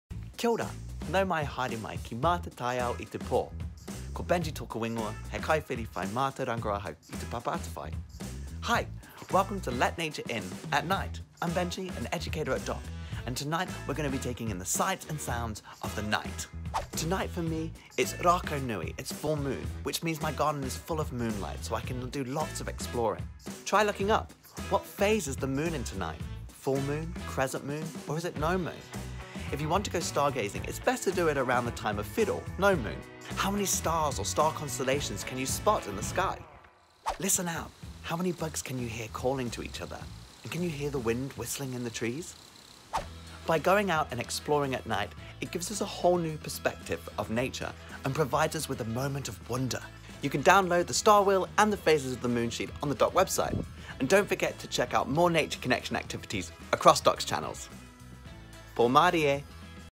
Let Nature In — Benji's Star Wheel
Nature doesn't stop once the sun goes down.

Under the light of the Rākaunui (full moon), Benji shares this week's Let Nature In activity: the Star Wheel.

Exploring at night offers a whole new perspective on nature and the wonders it provides us.

You can download your very own star wheel and the phases of the moon sheet on the DOC